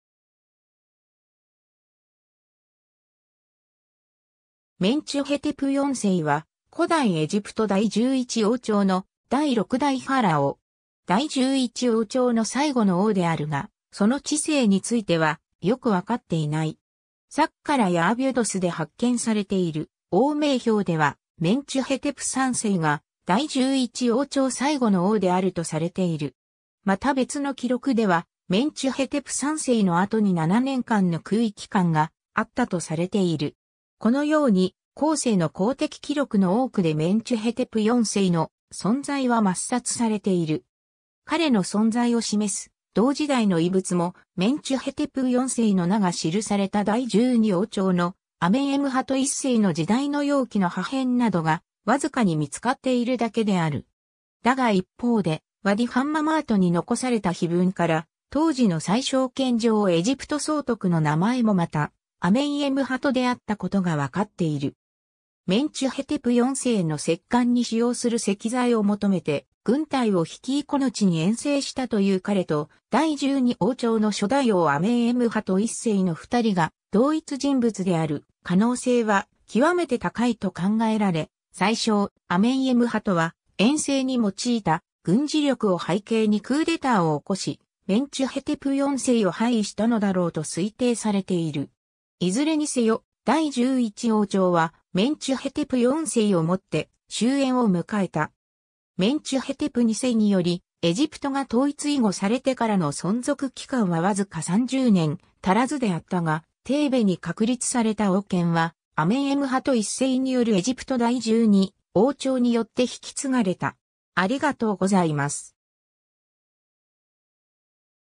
メンチュヘテプ4世
メンチュヘテプ4世（Mentuhotep IV, 在位：紀元前1998年 - 紀元前1991年）は、古代エジプト第11王朝の第6代ファラオ（王）。
 第11王朝の最後の王であるが、その治世についてはよく分かっていない。
サッカラやアビュドスで発見されている王名表ではメンチュヘテプ3世が第11王朝最後の王であるとされている。
また別の記録ではメンチュヘテプ3世の後に7年間の空位期間があったとされている。
このように、後世の公的記録の多くでメンチュヘテプ4世の存在は抹殺されている。
 彼の存在を示す同時代の遺物も、メンチュヘテプ4世の名が記された第12王朝のアメンエムハト1世の時代の容器の破片などがわずかに見つかっているだけである。
だが一方で、ワディ・ハンママートに残された碑文から、当時の宰相兼上エジプト総督の名前もまた「アメンエムハト」であったことがわかっている。
メンチュヘテプ4世の石棺に使用する石材を求めて軍隊を率いこの地に遠征したという彼と、第12王朝の初代王アメンエムハト1世の二人が同一人物である可能性は極めて高いと考えられ、宰相アメンエムハトは遠征に用いた軍事力を背景にクーデターを起こし、メンチュヘテプ4世を廃位したのだろうと推定されている。
 いずれにせよ、第11王朝はメンチュヘテプ4世をもって終焉を迎えた。
メンチュヘテプ2世によりエジプトが統一以後されてからの存続期間はわずか30年足らずであったが、テーベに確立された王権はアメンエムハト1世によるエジプト第12王朝によって引き継がれた。